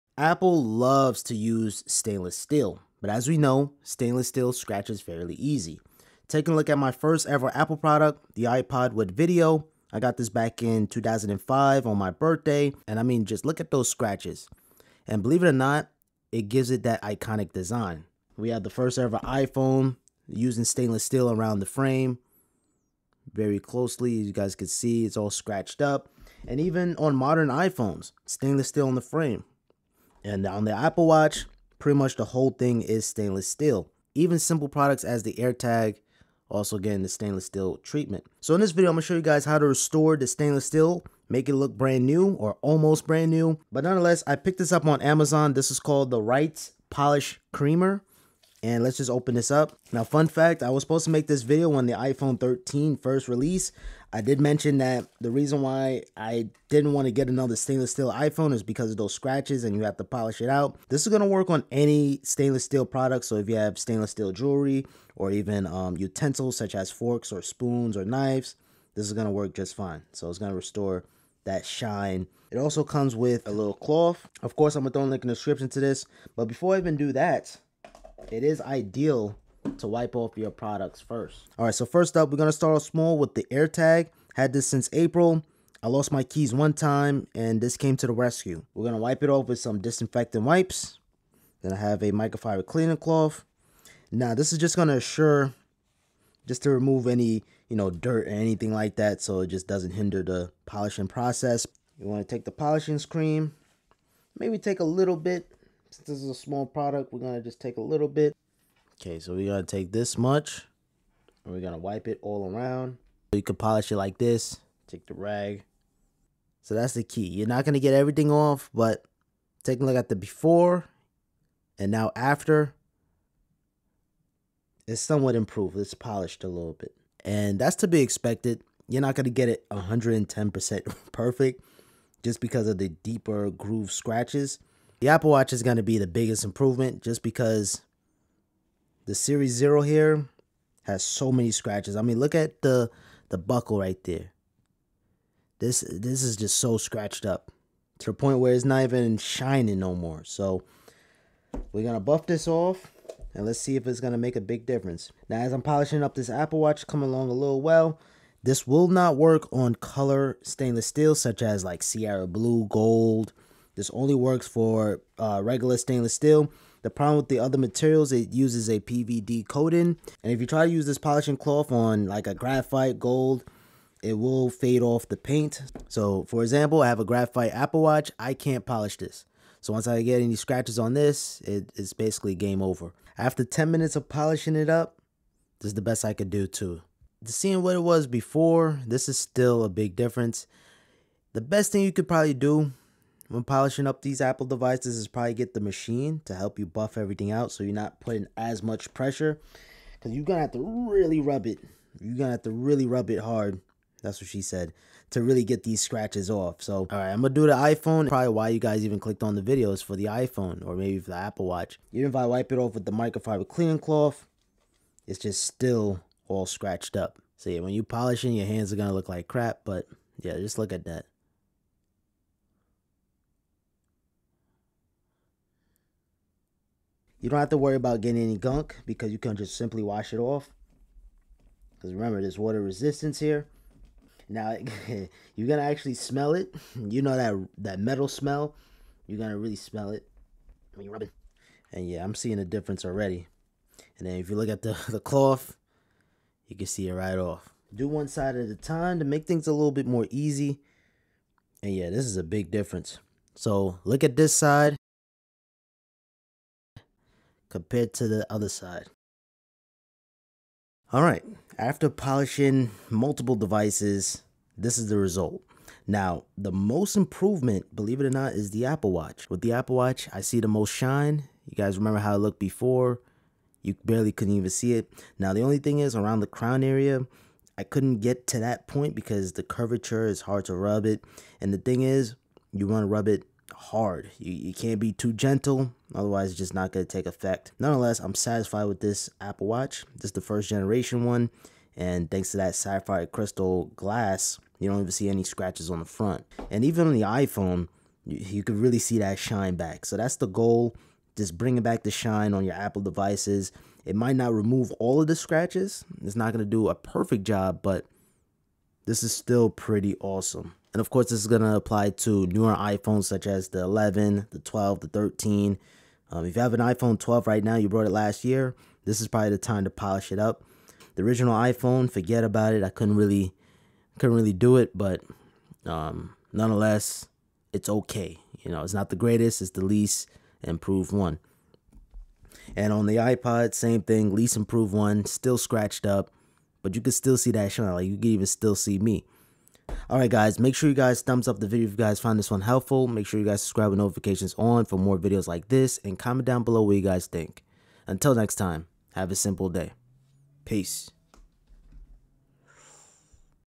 Remove Scratches on iPhone Stainless Steel - Polish Back to Shine
Bring back the shine on Apple devices.

Polishing Cream:

GEAR
Camera Lens
Camera Mic
SD Card
FREE Amazon Prime Trial